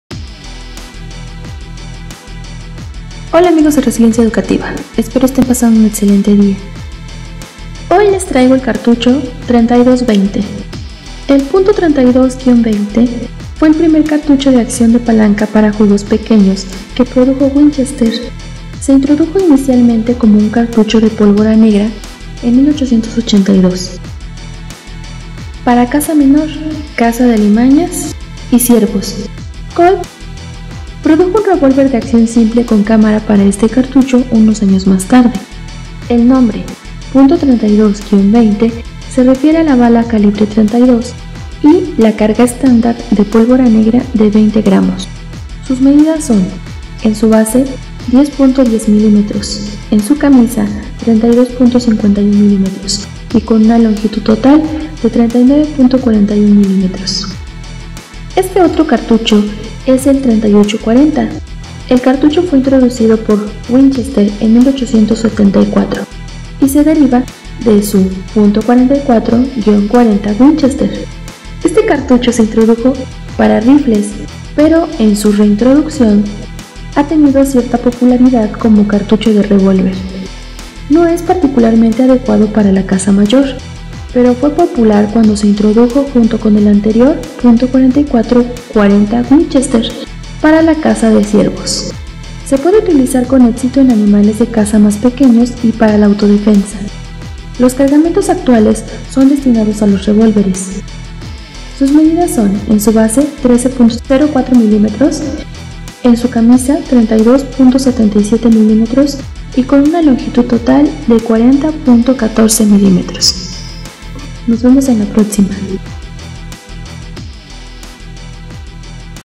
Cartuchos 32-20 y 38-40
En este video describiremos las características de los Cartuchos 32-20 y 38-40.